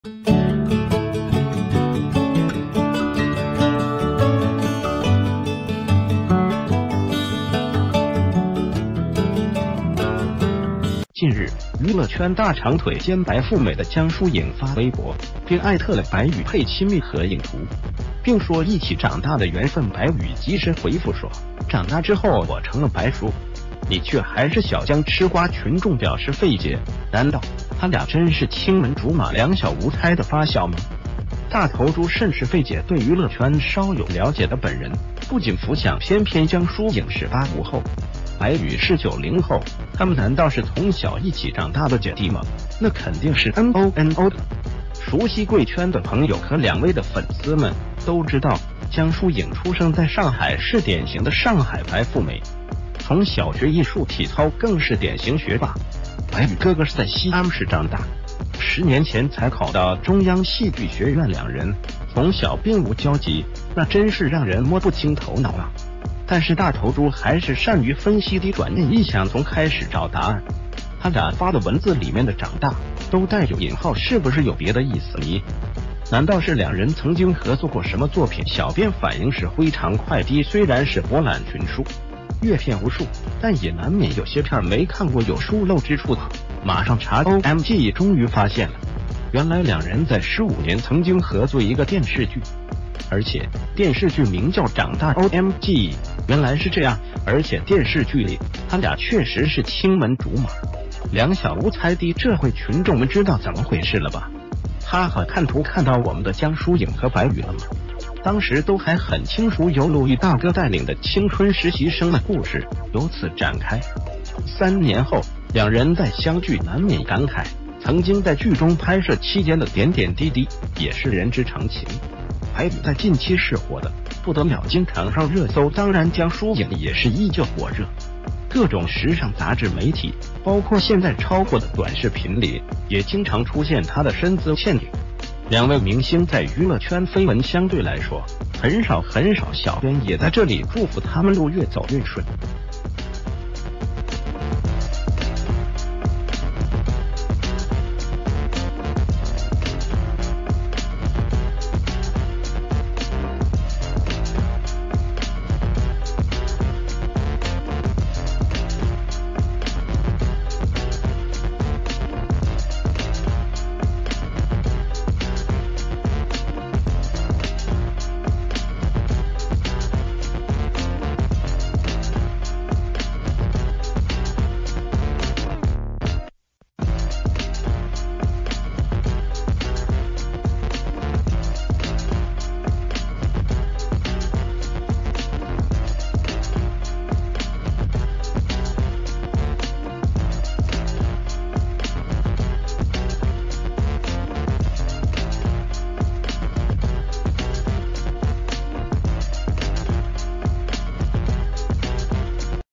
江疏影和白宇是發小？還是白宇最近超火，江疏影蹭熱度？
谢谢！

     近日，娛樂圈大長腿兼白富美的江疏影發微博并艾特了白宇配親密合影圖，并說一起“長大”的緣分。白宇及時回復說：“長大”之后，我成了白叔，你卻還是小江。
    吃瓜群眾表示費解，難道他倆真是青門竹馬，兩小無猜的發小嗎？
     大頭豬甚是費解。對娛樂圈稍有了解的本人，不僅浮想翩翩，江疏影是八五后，白宇是九零后，他們難道是從小一起長大的姐弟嗎？那肯定是NO NO了，熟悉貴圈的朋友和兩位的粉絲們都知道，江疏影出生在上海，是典型的上海白富美，從小學藝術體操，更是典型學霸；白宇哥哥是在西安市長大，十年前才考到中央戲劇學院，兩人從小并無交集。
    那真是讓人摸不清頭腦啊。但是大頭豬還是善于分析滴。轉念一想，從開始找答案，他倆發的文字里面的長大都帶有引號，是不是有別的意思尼。難道是兩人曾經合作過什么作品。小編反應是灰常快滴。雖然是博覽群書，閱片無數，但也難免有些片兒沒看過，有疏漏之處啊！馬上查！OMG，終于發現了，原來兩人在15年曾經合作一個電視劇，而且電視劇名叫長大，OMG！原來是這樣！而且電視劇里，他倆確實是青門竹馬，兩小無猜滴！這會群眾們知道怎么回事了吧!哈哈！看圖
    看到我們的江疏影和白宇了么，當時都還很輕熟，由陸毅大哥帶領的青春實習生的故事由此展開。三年后兩人再相聚，難免感慨曾經在劇中拍攝期間的點點滴滴，也是人之常情。白宇在近期是火的不得了，經常上熱搜，當然江疏影也是依舊火熱，各種時尚雜志媒體，包括現在超火的短視頻里也經常出現她的身姿倩影。兩位明星在娛樂圈緋聞相對來說很少很少，小編也在這里祝福他們路越走越順，越來越紅唄！